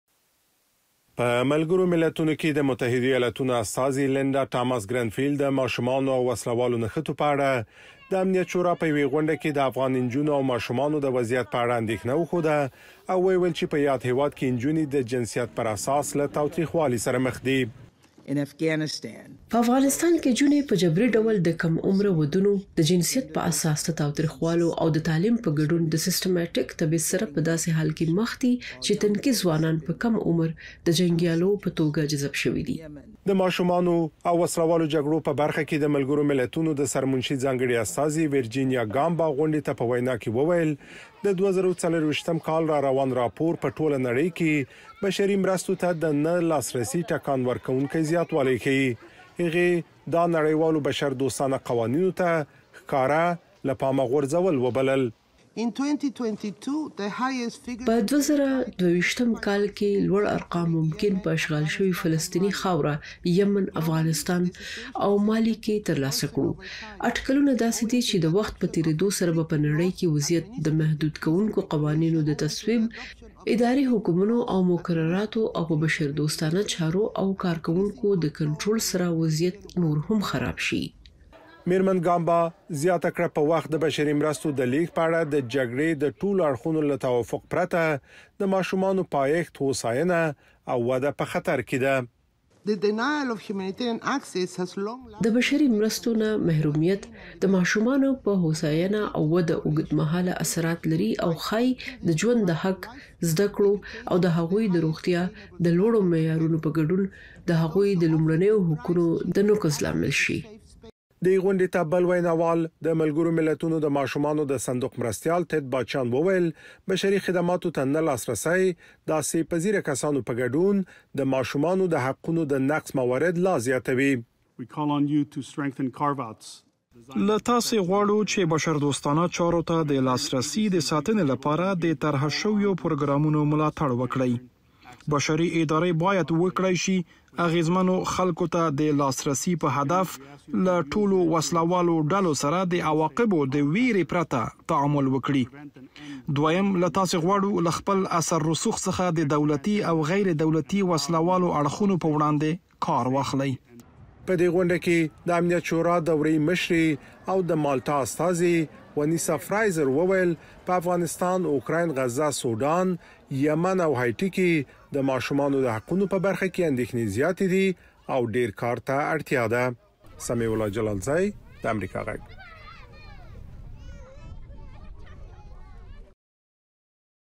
د ماشومانو په رواني روغتیا د نړۍ په بیلابیلو هیوادونو کې د روانو جګړو اغیز
د ملګرو ملتونو سازمان وایي د نړۍ په ټولو هغو برخو کې چې جګړه روانه ده د ماشومانو حقونه تر پښو لاندې کیږي. د ماشومانو او وسله والو جګړو په برخه کې د ملګرو ملتونو د سرمنشي ځانګړې استازې ویرجینیا ګامبا د امنیت شورا غونډي ته په وینا کې وویل، ماشومانو ته د مرستو نه لاسرسی په اوږده مهال کې د هغوی پر ودې ناوړه تاثیر کوي. بلخوا په ملګرو ملتونو کې د متحدو ایالتونو استازي لنډا ټاماس ګرنفیلډ وویل چې افغانستان کې نجونې د جنسیت پر اساس له تاوتریخوالي سره مخ دي